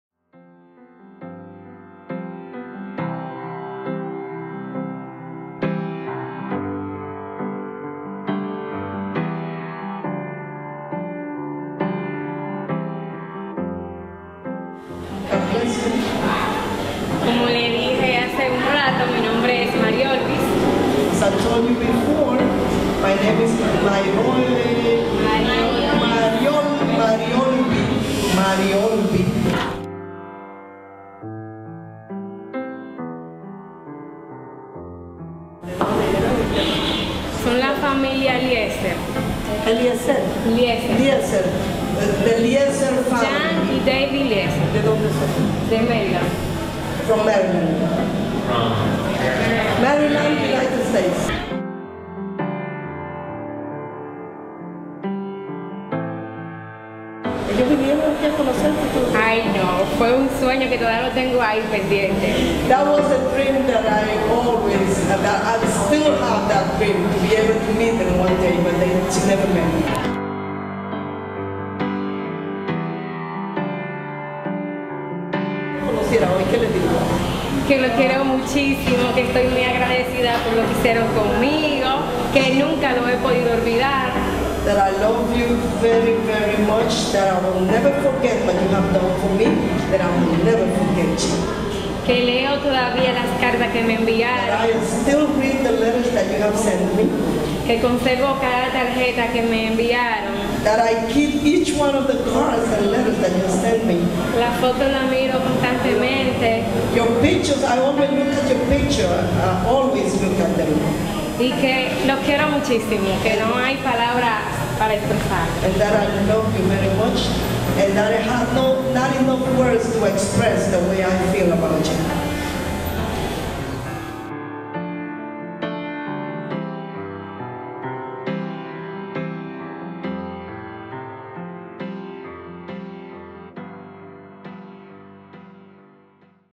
Three Questions For Mariolvis
Nov. 2-7, 2008 Compassion International took 6 bloggers to the Dominican Republic to see their ministry to children up close and write about it.

On the third day of the trip, the bloggers met Mariolvis.  Mariolvis was a sponsored child through Compassion International for many years.  Today she is a healthy confident 21 year-old attending the University of Santo Domingo.  She told the bloggers how Compassion International and her sponsor released her from poverty.  This is part of that story.

For more stories and blog posts from this blogging trip to the Dominican Republic,